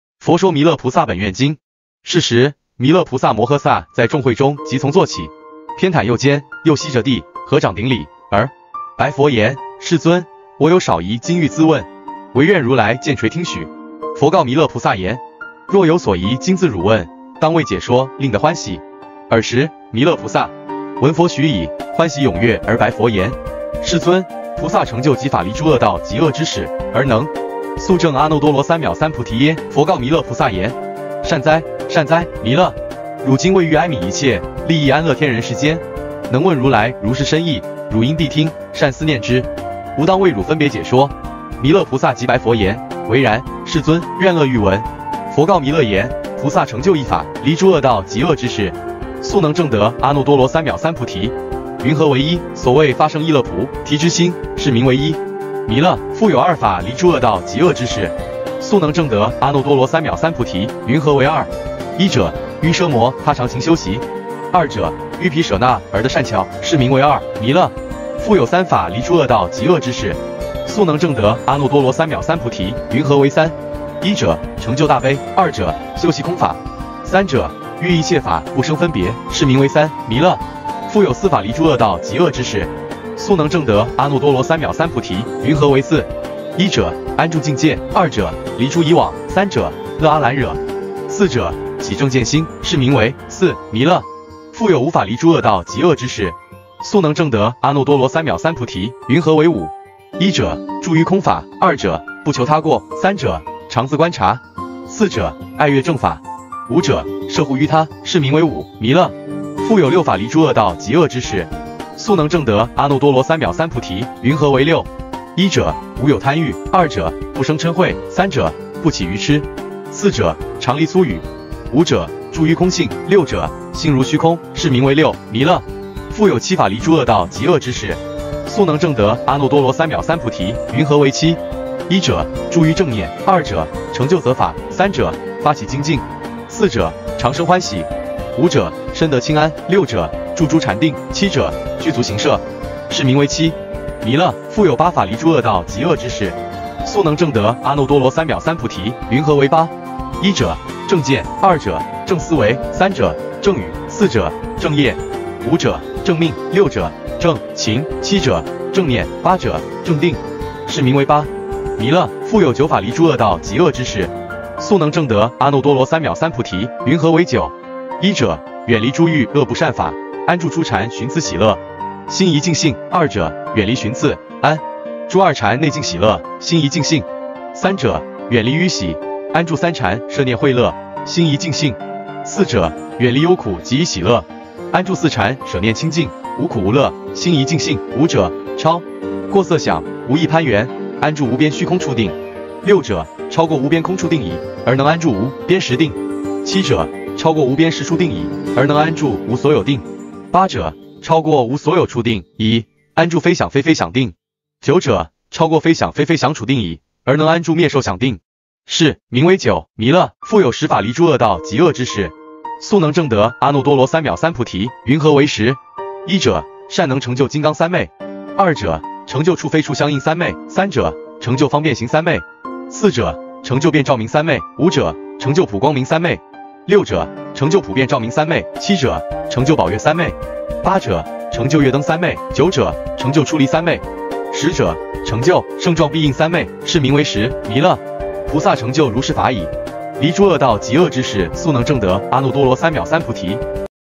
【佛說彌勒菩薩本願經】
是時，彌勒菩薩摩訶薩在眾會中即從座起，偏袒右肩，右膝著地，合掌頂禮，而白佛言：
「世尊，我有少疑今欲諮問，唯願如來見垂聽許。」
佛告彌勒菩薩言：
「若有所疑今恣汝問，當為解說令得歡喜。」爾時，彌勒菩薩聞佛許已，歡喜踊躍而白佛言：
「世尊，菩薩成就幾法離諸惡道及惡知識，而能速證阿耨多羅三藐三菩提耶？」
佛告彌勒菩薩言：
「善哉，善哉！彌勒！汝今為欲哀愍一切，利益安樂天人世間，能問如來如是深義，汝應諦聽，善思念之！吾當為汝分別解說。」
彌勒菩薩即白佛言：
「唯然，世尊。願樂欲聞。」

佛告彌勒言：
「菩薩成就一法離諸惡道及惡知識，速能證得阿耨多羅三藐三菩提。云何為一？所謂發勝意樂菩提之心，是名為一。

彌勒！復有二法離諸惡道及惡知識，速能證得阿耨多羅三藐三菩提。云何為二？
一者、於奢摩他常勤修習。
二者、於毘舍那而得善巧。
是名為二。
彌勒！復有三法離諸惡道及惡知識，速能證得阿耨多羅三藐三菩提。
云何為三？
一者、成就大悲。
二者、修習空法。
三者、於一切法不生分別。
是名為三。
彌勒！復有四法離諸惡道及惡知識，速能證得阿耨多羅三藐三菩提。
云何為四？
一者、安住淨戒。
二者、離諸疑網。
三者、樂阿蘭若。
四者、起正見心。
是名為四。
彌勒！復有五法離諸惡道及惡知識，速能證得阿耨多羅三藐三菩提。
云何為五？
一者、住於空法。
二者、不求他過。
三者、常自觀察。
四者、愛樂正法。
五者、攝護於他。
是名為五。
彌勒！復有六法離諸惡道及惡知識，速能證得阿耨多羅三藐三菩提。
云何為六？
一者、無有貪欲。
二者、不生瞋恚。
三者、不起愚癡。
四者、常離粗語。
五者、住於空性。
六者、心如虛空。
是名為六。
彌勒！復有七法離諸惡道及惡知識，速能證得阿耨多羅三藐三菩提。
云何為七？
一者、住於正念。
二者、成就擇法。
三者、發起精進。
四者、常生歡喜。
五者、身得輕安。
六者、住諸禪定。
七者、具足行捨。
是名為七。
彌勒！復有八法離諸惡道及惡知識，速能證得阿耨多羅三藐三菩提。
云何為八？
一者、正見。
二者、正思惟。
三者、正語。
四者、正業。
五者、正命。
六者、正勤。
七者、正念。
八者、正定。
是名為八。
彌勒！復有九法離諸惡道及惡知識，速能證得阿耨多羅三藐三菩提。
云何為九？
一者、遠離諸欲惡不善法，安住初禪尋伺喜樂，心一境性。
二者、遠離尋伺，安住二禪內淨喜樂，心一境性。
三者、遠離於喜，安住三禪捨念慧樂，心一境性。
四者、遠離憂苦及以喜樂，安住四禪捨念清淨、無苦無樂，心一境性。
五者、超過色想，無異攀緣，安住無邊虛空處定。
六者、超過無邊空處定已，而能安住無邊識定。
七者、超過無邊識處定已，而能安住無所有定。
八者、超過無所有處定已，安住非想非非想定。
九者、超過非想非非想處定已，而能安住滅受想定。
是名為九。
彌勒！復有十法離諸惡道及惡知識，速能證得阿耨多羅三藐三菩提。
云何為十？
一者、善能成就金剛三昧。
二者、成就處非處相應三昧。
三者、成就方便行三昧。
四者、成就遍照明三昧。
五者、成就普光明三昧。
六者、成就普遍照明三昧。
七者、成就寶月三昧。
八者、成就月燈三昧。
九者、成就出離三昧。
十者、成就勝幢臂印三昧。
是名為十。
彌勒！菩薩成就如是法已，離諸惡道及惡知識速能證得阿耨多羅三藐三菩提。」